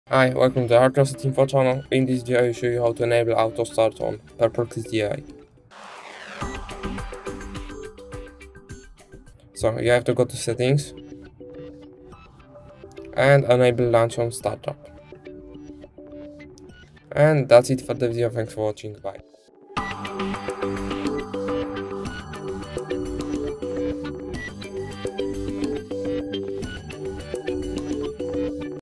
Enable Autostart in Perplexity AI
Hey! Discover how to enable autostart in Perplexity AI so it launches automatically when you need it. This is great for frequent users who want quick access.

How do I turn on autostart?
Can Perplexity open automatically?
Where is the autostart setting?